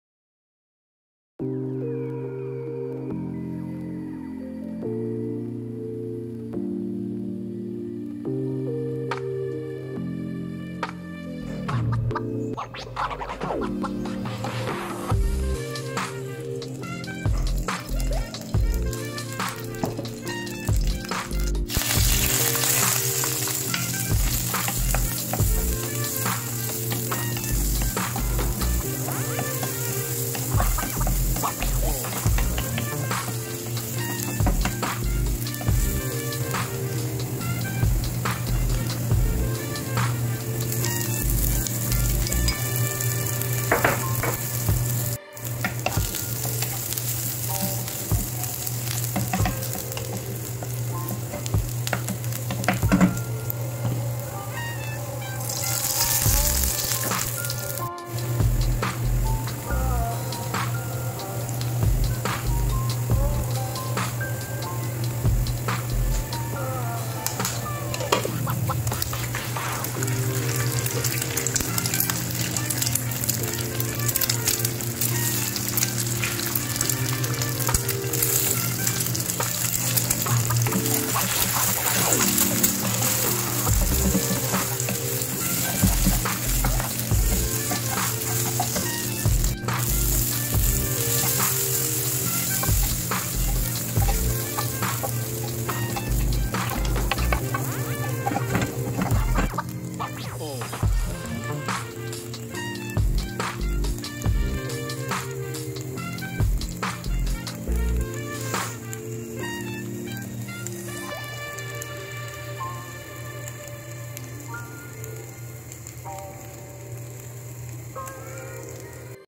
If u have 2 eggs and Onions then make this delicious breakfast recipie in 2 minutes|Eggs Hosh Posh
Eggs onion hosh posh